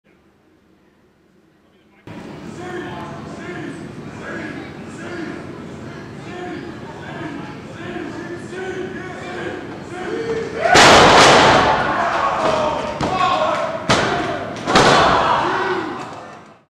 El Predator - Mass Anarchy Wrestling
Here is El Predator doing a high risk maneuver on DeathStrike at the Mass Anarchy Wrestling show called "Unscripted 2". This took place in West Warwick, Rhode Island.